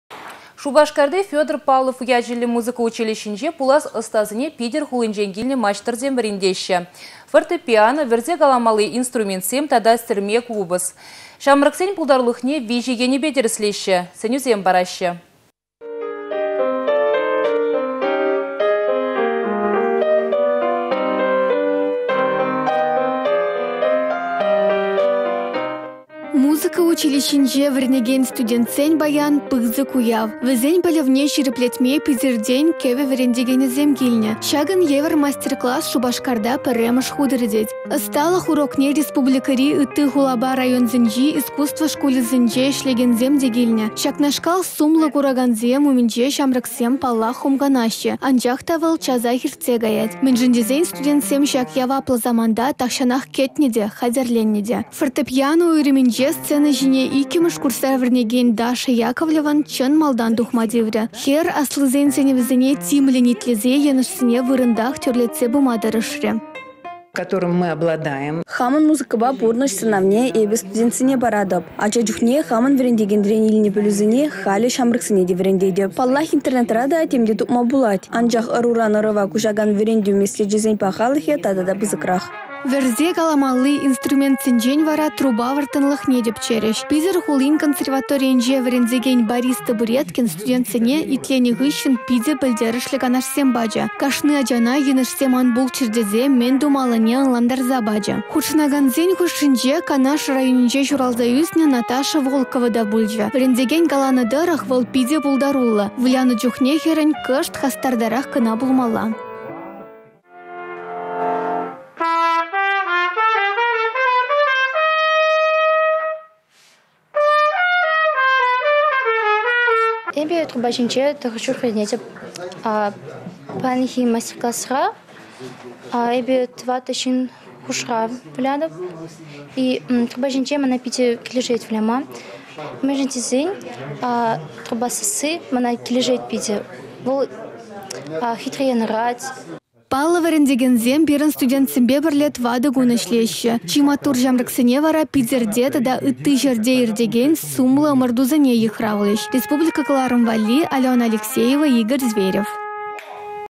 Питĕр хулинчен килнĕ маçтăрсем вĕрентеççĕ.
Шупашкарти Федор Павлов ячĕллĕ музыка училищинче пулас ăстасене Питĕр хулинчен килнĕ маçтăрĕсем вĕрентеççĕ. Фортепиано, вĕрсе каламалли инструментсем тата сĕрме купăс. Çамрăксен пултартулăхне виçĕ енĕпе тĕрĕслеççĕ, сĕнÿсем параççĕ.